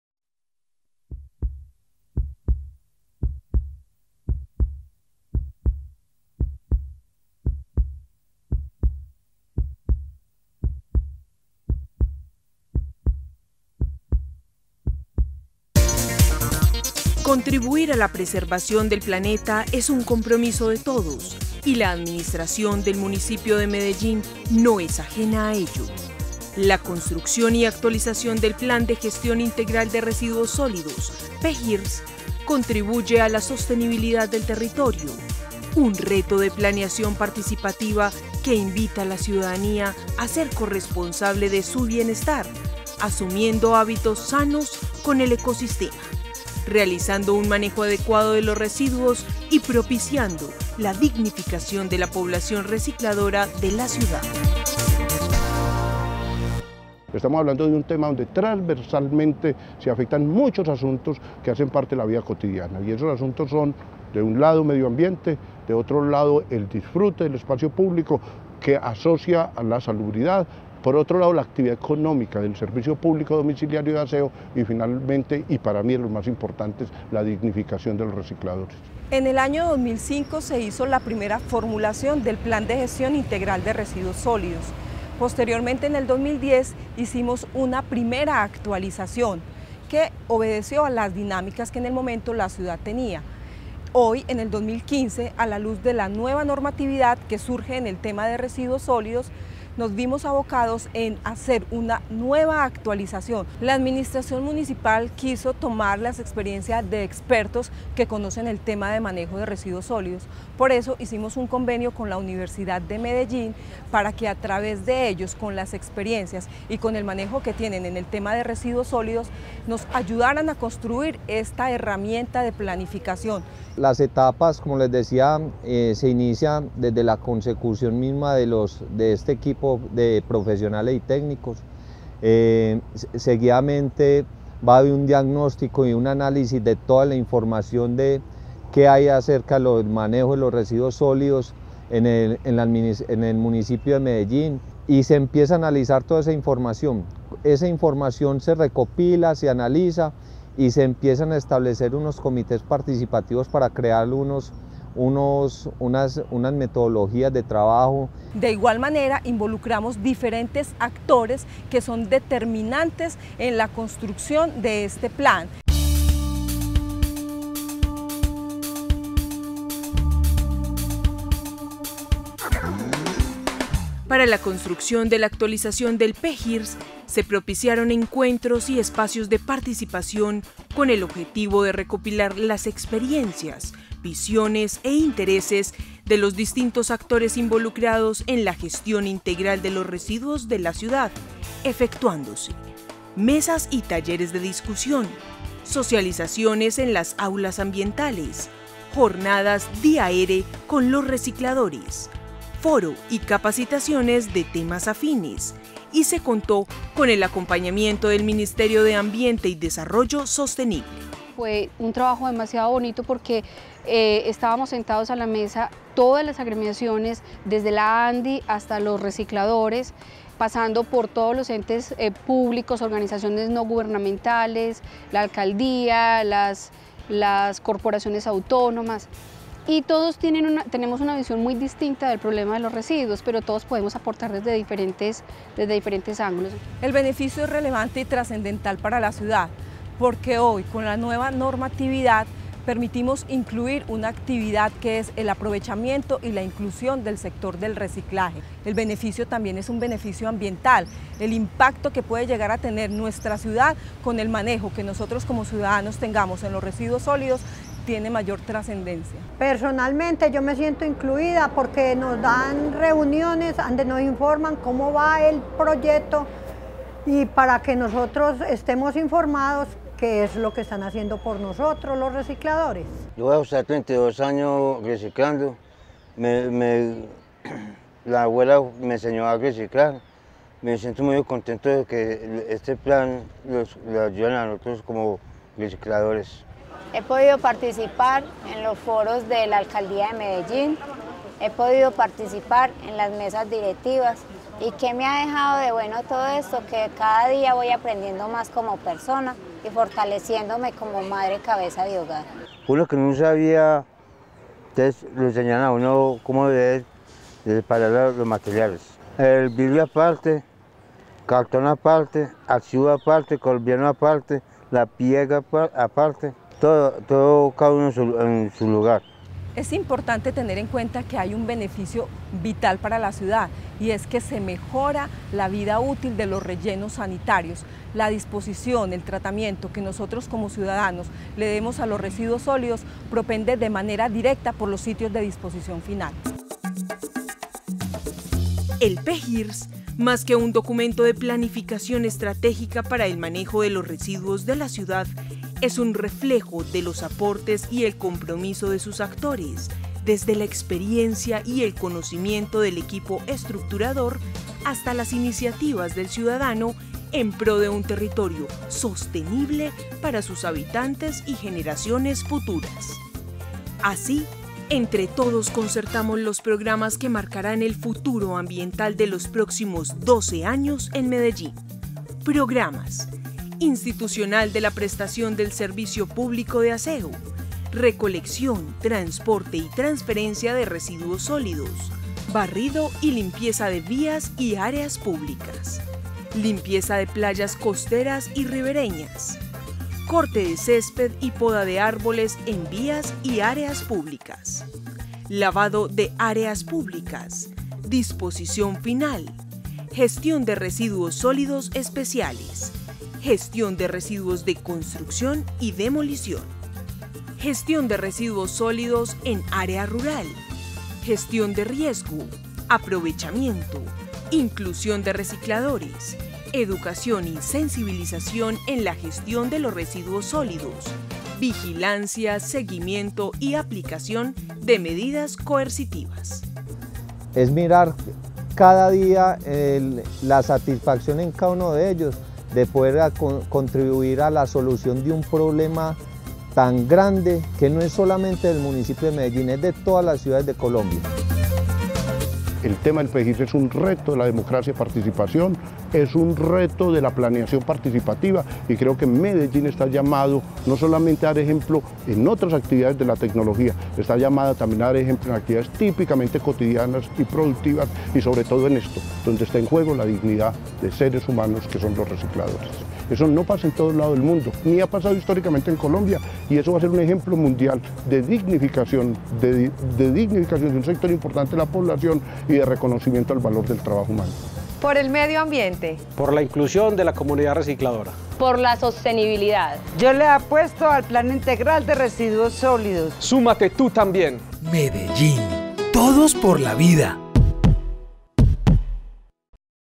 Así fue la actualización del Plan de Residuos Sólidos (PGIRS) de la Alcaldía de Medellín
En este video te explicamos cómo fue el proceso de la actualización del Plan de Residuos Sólidos (PGIRS) de la Alcaldía de Medellín y sus resultados. Este plan contribuirá en la sostenibilidad de Medellín durante los próximos 12 años.